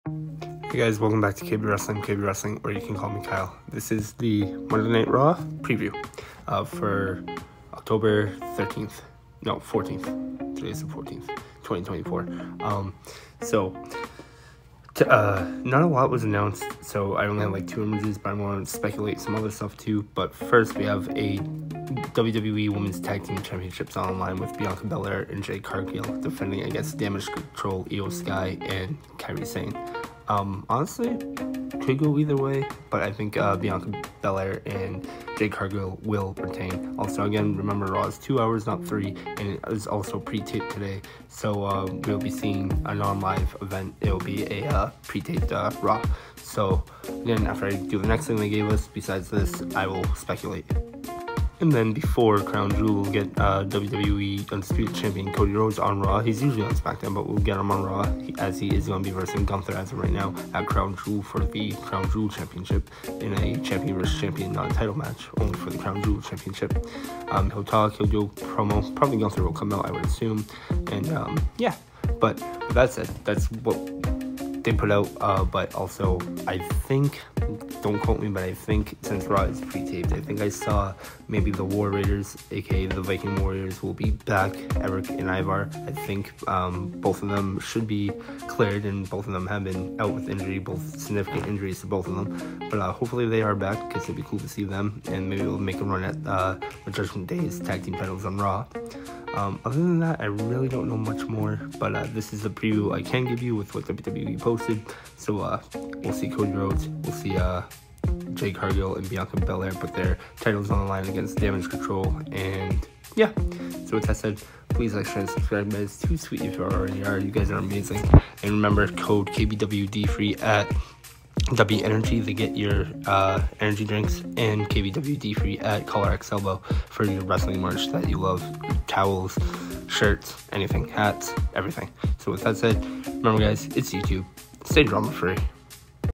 WWE RAW Preview for 10/14/2024! Cody Rhodes to Appear on RAW, & WWE Woman’s Tag Titles on the Line!
Preview for tonight’s episode of WWE Monday Night RAW!